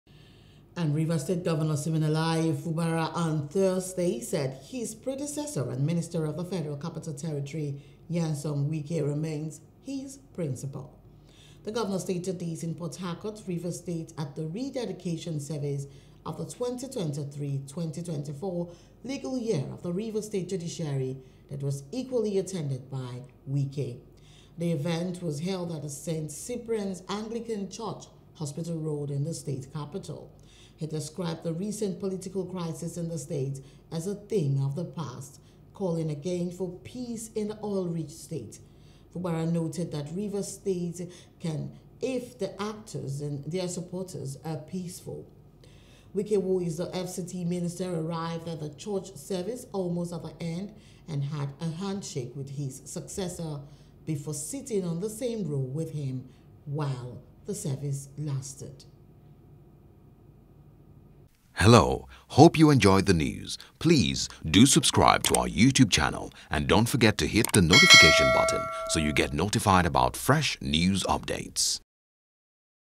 Wike Remains My Principal - Governor Fubara
Rivers State Governor, Siminalayi Fubara, on Thursday said his predecessor and Minister of the Federal Capital Territory, Nyesom Wike remains his principal.